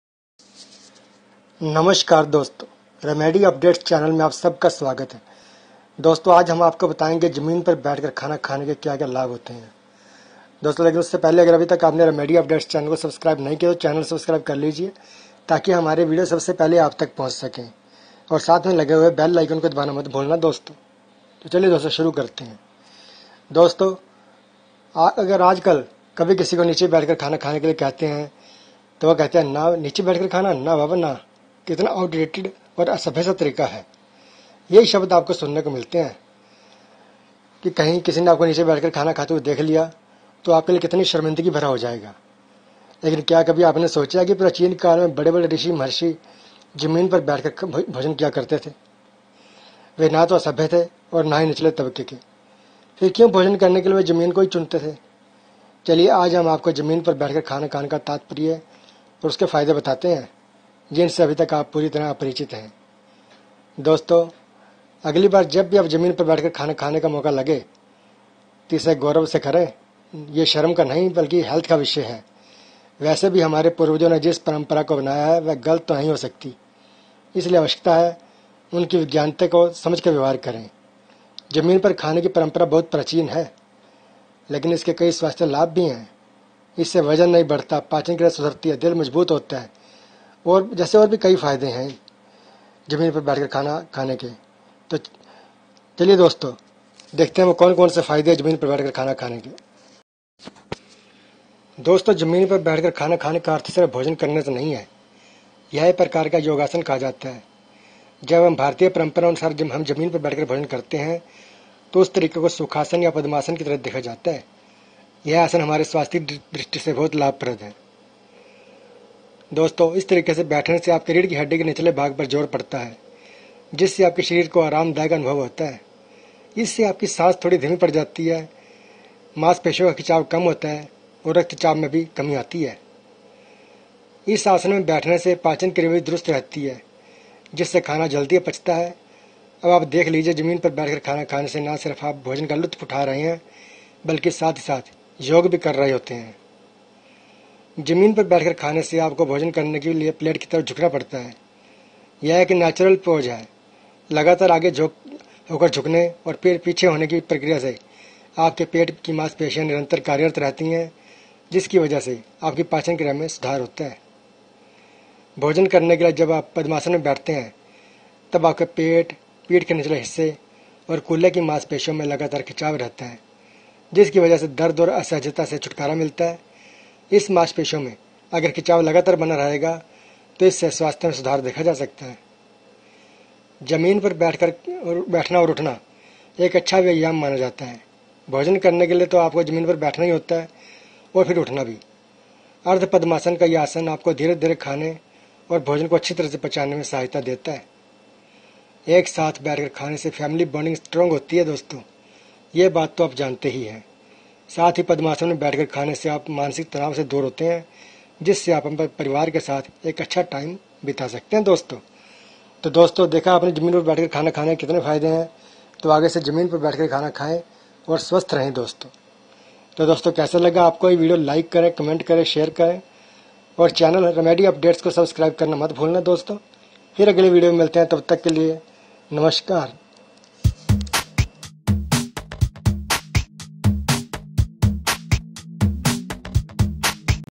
क्यों खाना जमीन पर बैठ कर खाना चाहिए, जाने क्या है लाभ जमीन पर बैठ कर खाने के..!! REMEDY UPDATES !!
क्यों खाना जमीन पर बैठ कर खाना चाहिए, जाने क्या है लाभ जमीन पर बैठ कर खाने के
नीचे बैठकर खाना!! ना बाबा ना, कितना आउटडेटेड और असभ्य सा तरीका है…..” जब कभी कोई आपको नीचे बैठकर खाने के लिए कहता है तो आपके दिमाग में यही शब्द घूमते होंगे कि कहीं किसी ने आपको नीचे बैठकर खाना खाते हुए देख लिया तो ये आपके लिए कितना शर्मिंदगी भरा होगा। लेकिन क्या कभी आपने सोचा है कि प्राचीन काल में बड़े-बड़े ऋषि-महर्षि जमीन पर बैठकर ही भोजन क्यों किया करते थे? वे ना तो असभ्य थे और ना ही निचले तबके के, फिर क्यों भोजन करने के लिए वे भूमि को ही चुनते थे? चलिए आज हम आपको जमीन पर बैठकर खाने का तात्पर्य और उसके फायदे बताते हैं, जिनसे अभी तक आप पूरी तरह अपरिचित हैं। इन्हें ध्यान से पढ़ें और अगली बार से जब जमीन पर बैठकर खाना खाने का मौका लगे तो इसे गौरव से करें। ये शर्म का नहीं हेल्थ का विषय है। वैसे भी हमारे पूर्वजों ने जिस परंपरा को बनाया है, वह गलत तो नहीं हो सकती इसलिए आवश्यकता है कि उनकी वैज्ञानिकता को समझकर व्यवहार करें।

जमीन पर खाने की परंपरा बहुत प्राचीन है, लेकिन इसके कई स्‍वास्‍थ्‍य लाभ भी हैं, इससे वजन नहीं बढ़ता, पाचन क्रिया सुधरती है, दिल मजबूत होता है जैसे कई फायदे हैं इसके, तो जमीन पर खाने की आदत डालें।